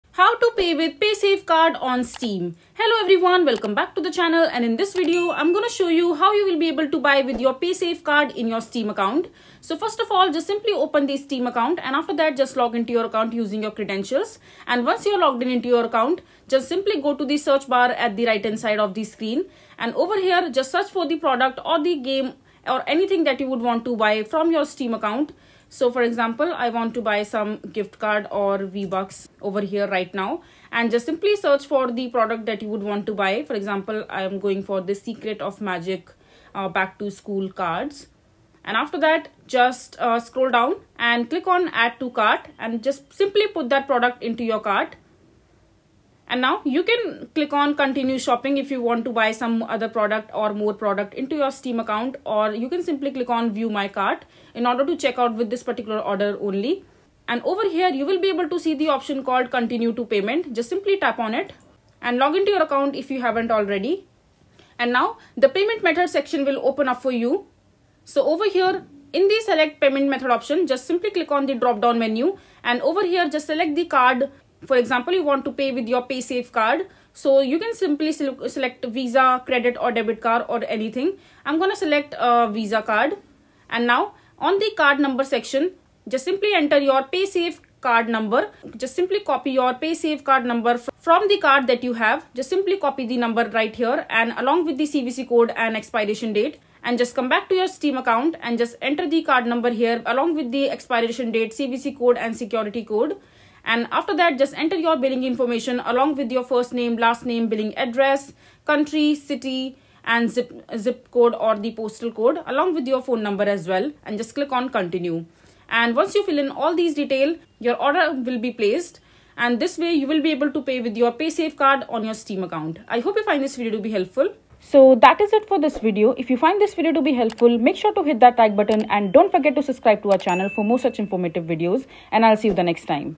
How to Pay with Paysafecard on Steam (Full Guide)
Learn how to pay with Paysafecard on Steam with this full guide. Follow the step-by-step instructions to make your purchases hassle-free!